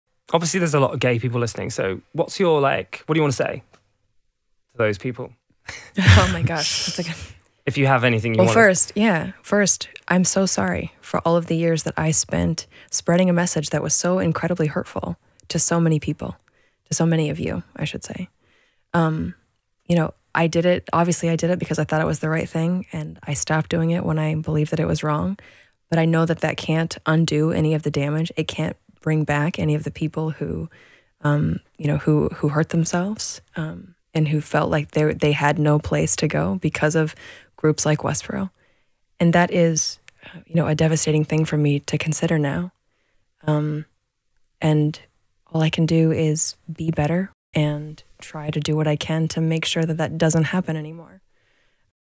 Megan Phelps-Roper says sorry to the LGBTQ+ community, on A Gay and A NonGay
The Westboro Baptist Church are responsible for amplifying a profoundly offensive and homophobic rhetoric of hate towards the queer community. After leaving the church, Megan Phelps-Roper has a message for LGBTQ+ people around the world. Listen to the full interview now, download the podcast and check out the full video interview coming soon

A Gay and A NonGay is an independent podcast from James Barr and Dan Hudson